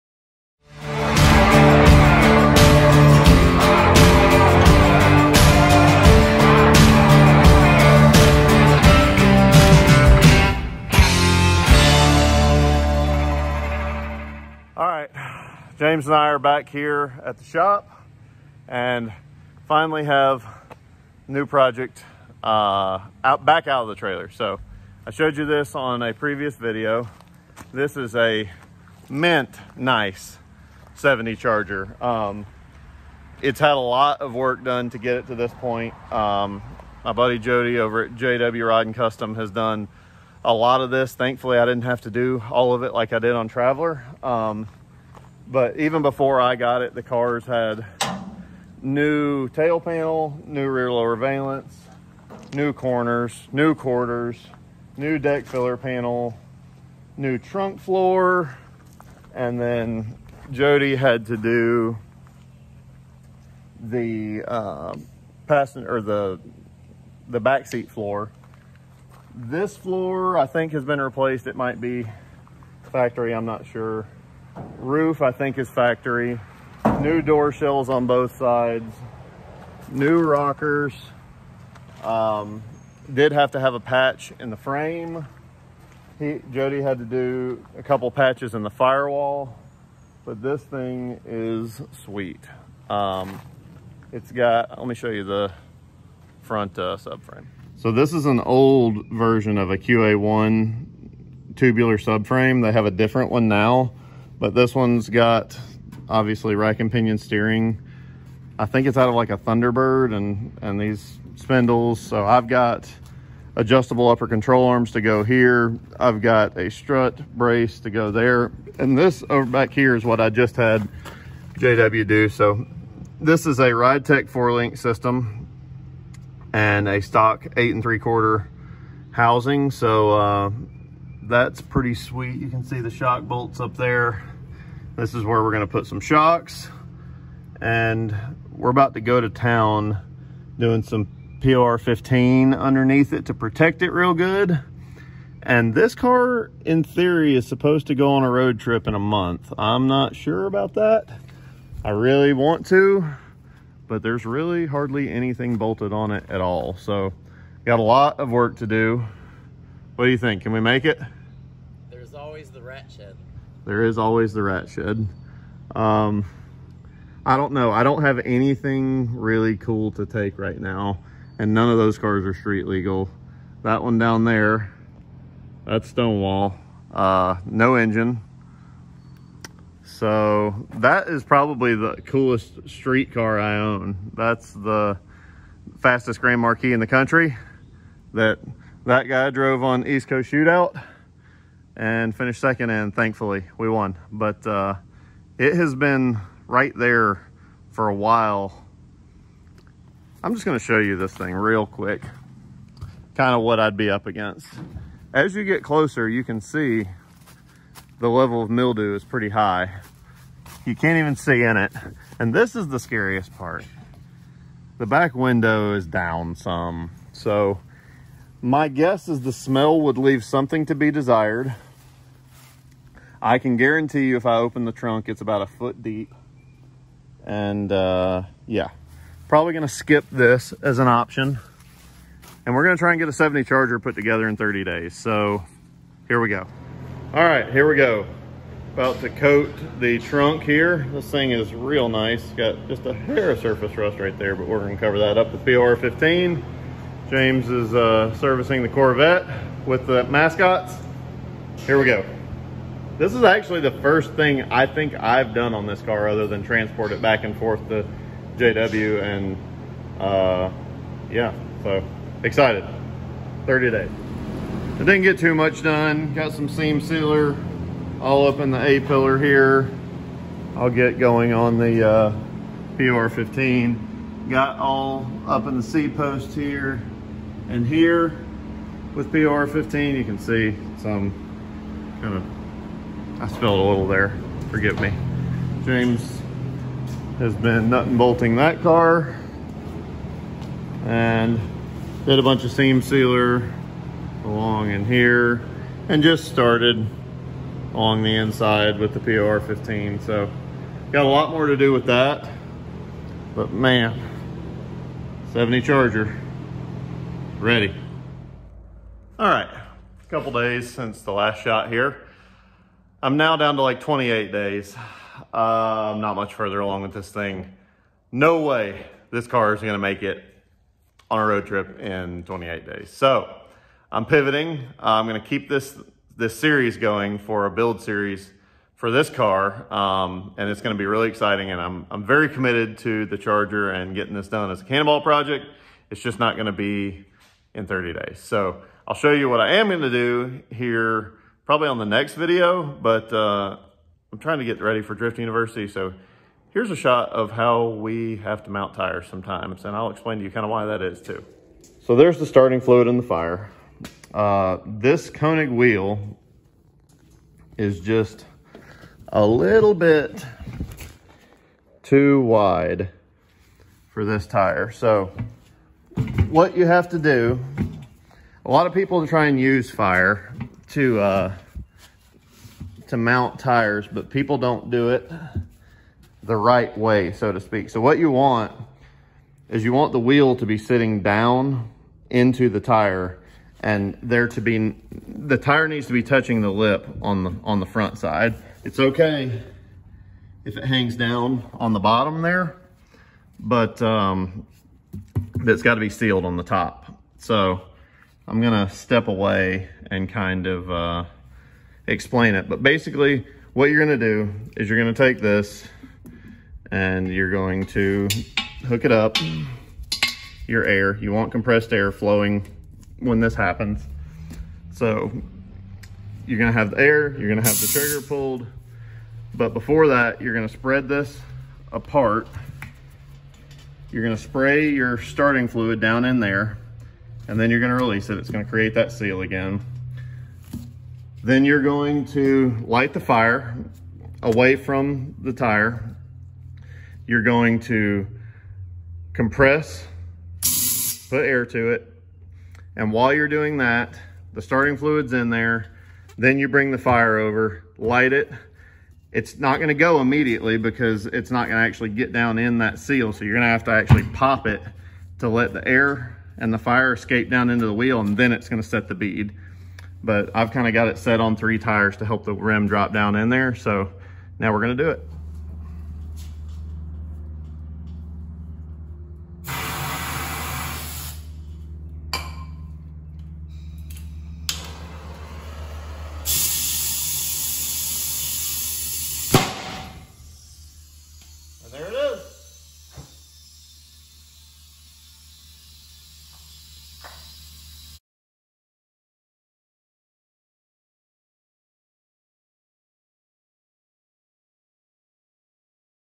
1970 Dodge Charger build part 1 + How to use tire to mount tires!
We’re building a 1970 Dodge Charger! In this video we go over what it is, some of what we’ll do with it, and even get a little paint down. Bonus! I show you how to mount tires when they just won’t bead!